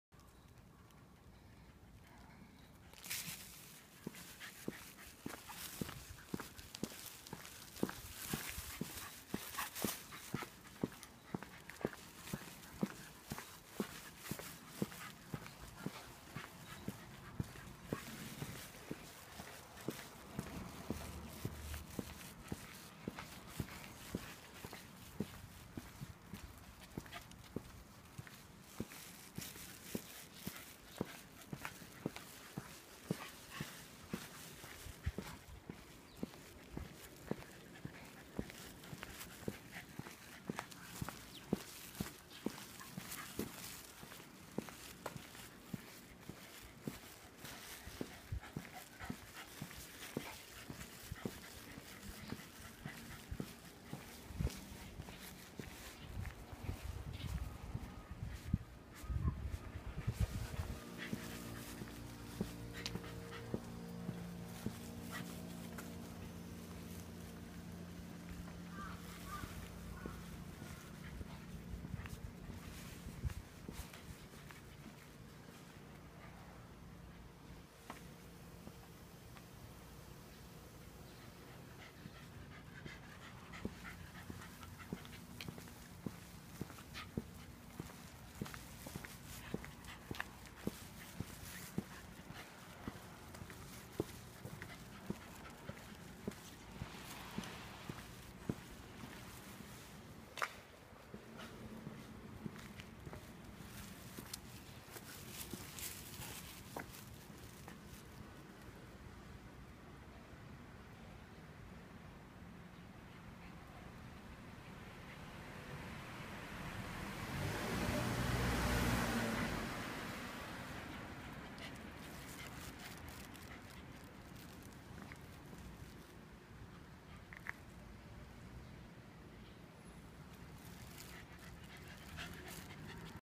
Shiba inu 柴犬スペック 20140527-02
尼崎市内の商店街西の端っこです。
カンナミ、座三和スタジオ、旧ダイエー三和店あと。